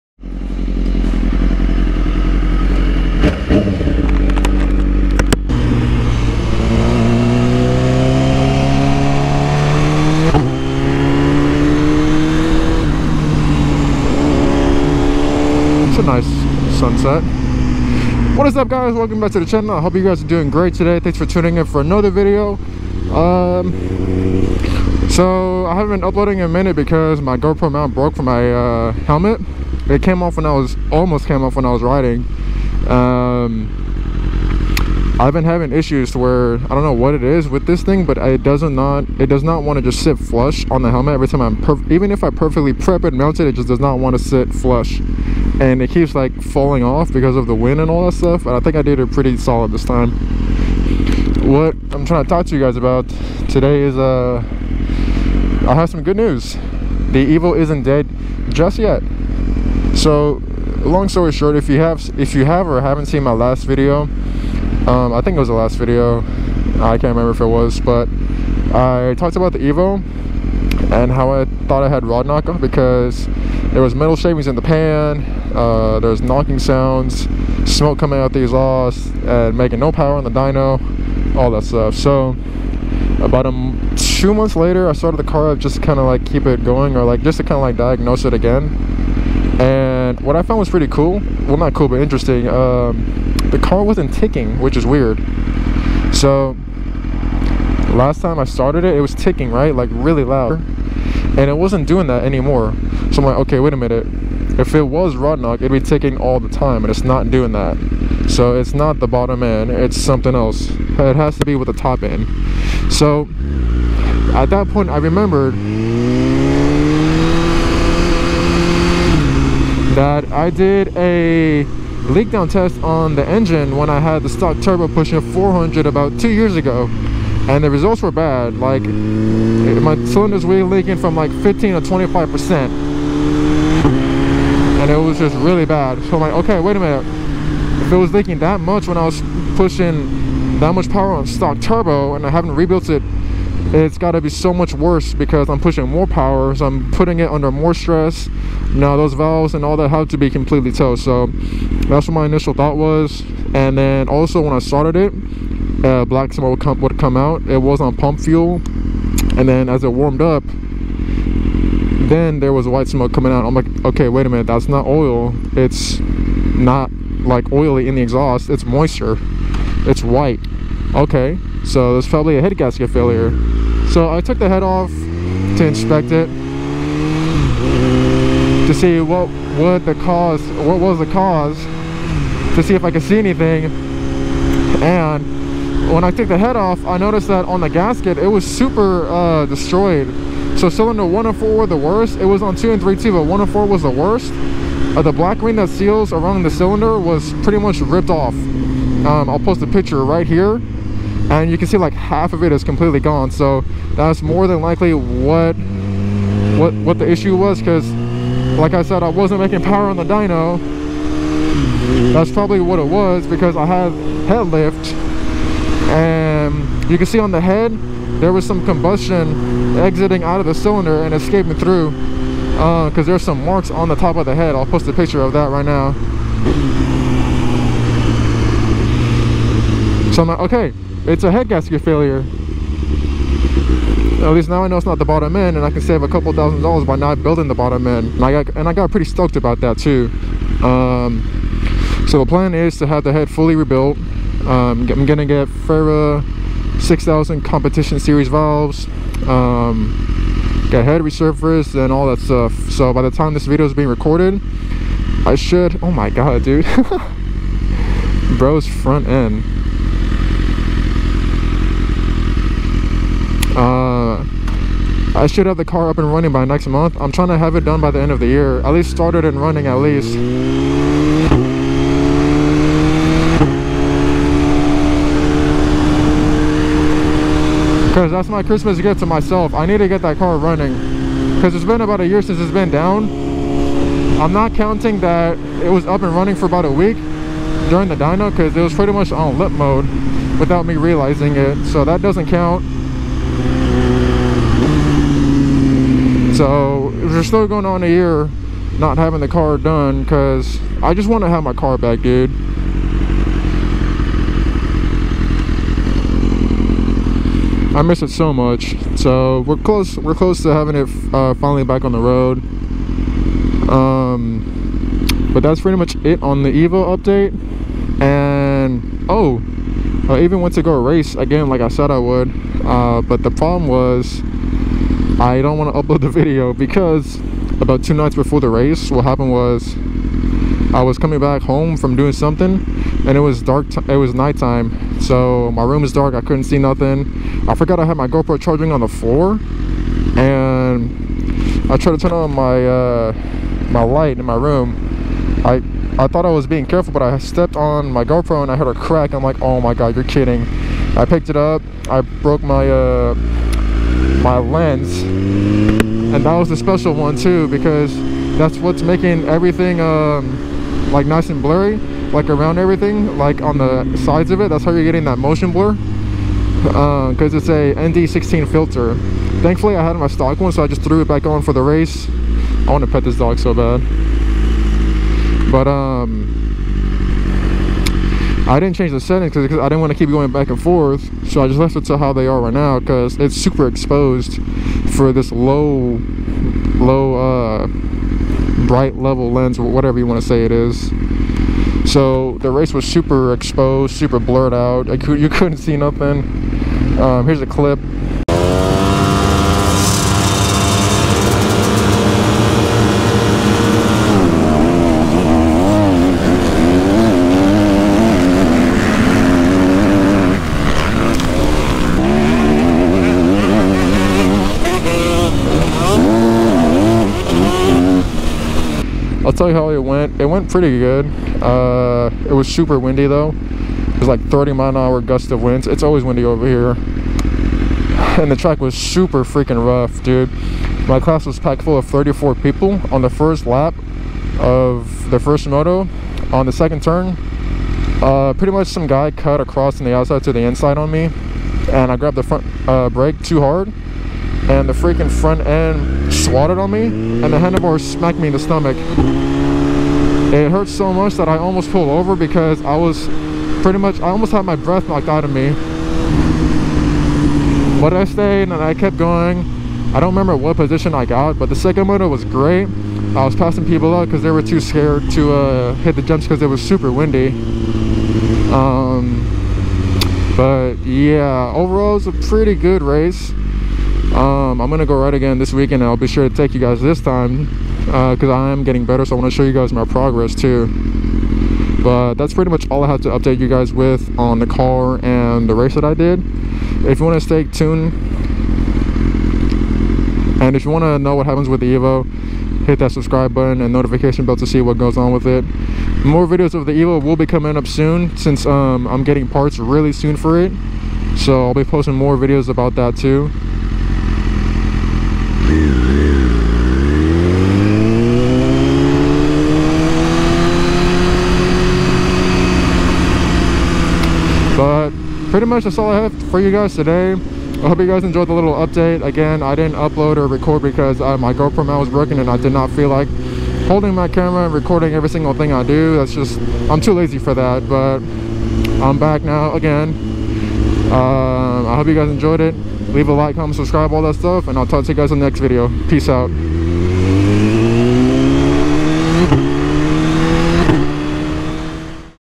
THE EVO ISN'T DEAD! yet
Giving you guys an update on the Evo, seems the engine is still savable and won't need a full rebiuld, should have it up and running by the end of this year.